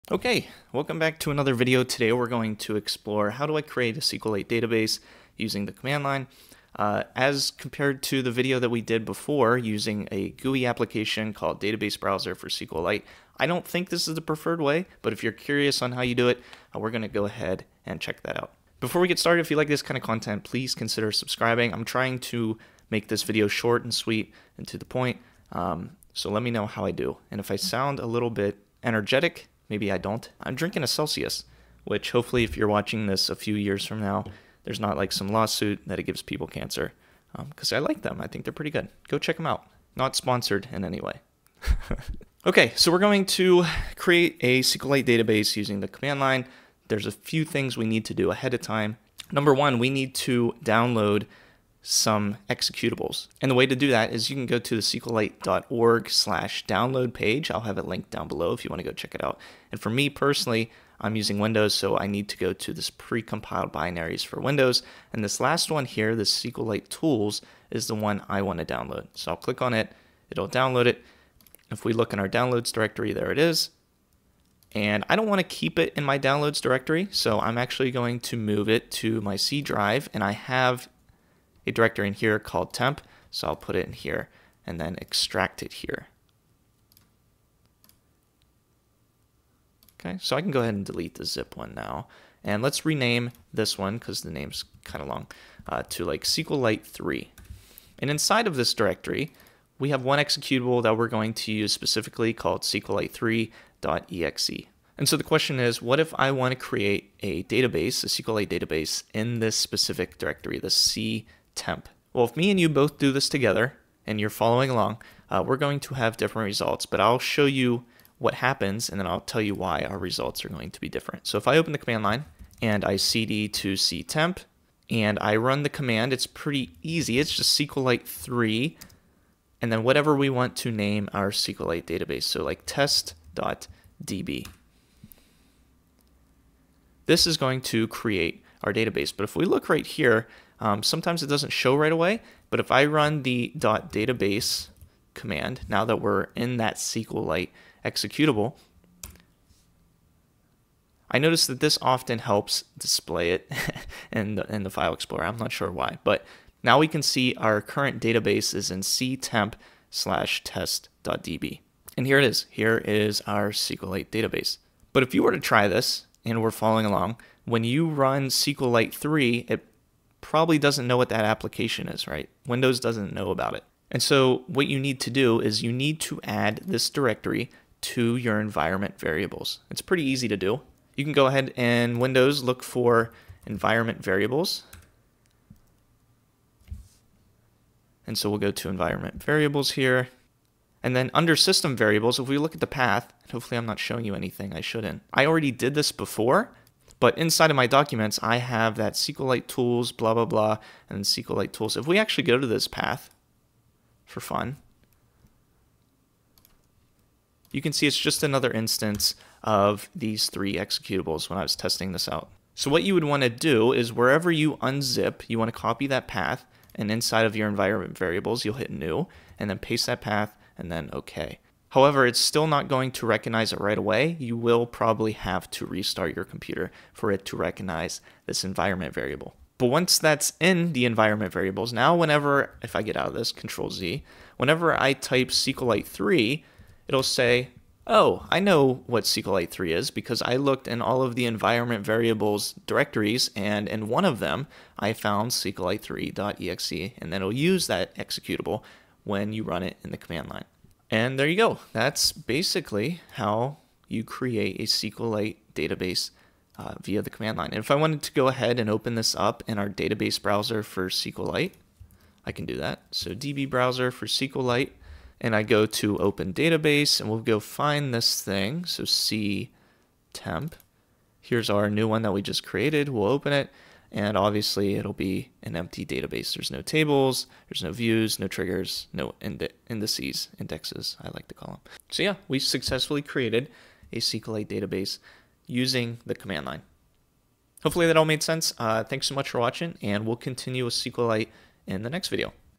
How to Create a SQLite Database with the Command Line in Windows
Today we are going to take a look at how to create a SQLite database using the command line and SQLite3.